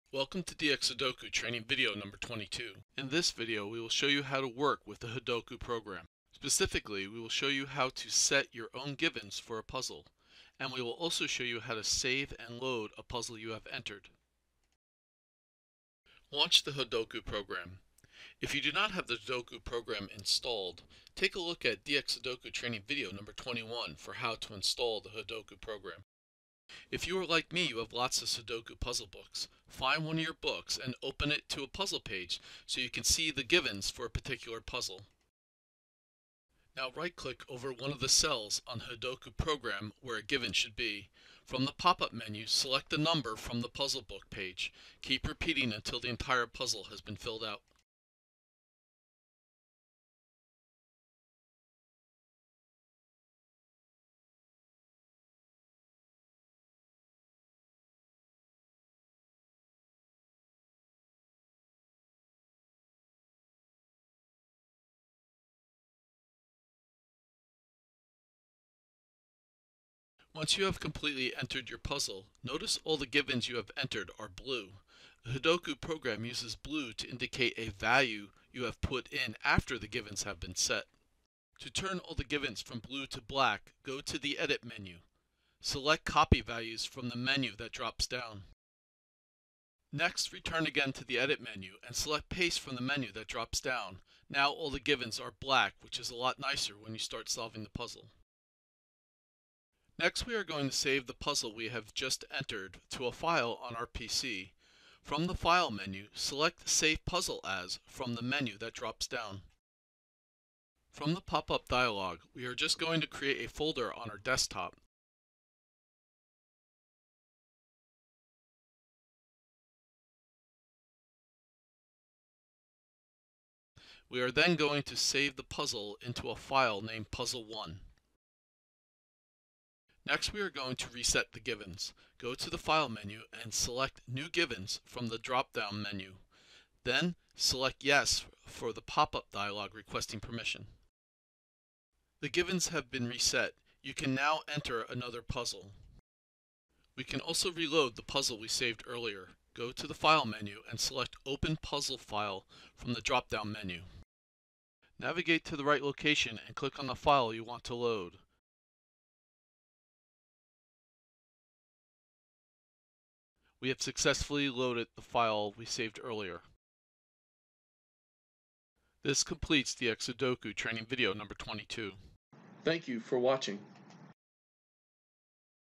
dxSudoku #22 Hodoku #2 Entering Your Own Givens, Saving and Loading
In this video we will show you how to enter givens into Hodoku from a puzzle outside the program.  And we will show you have to save and load puzzles you have entered.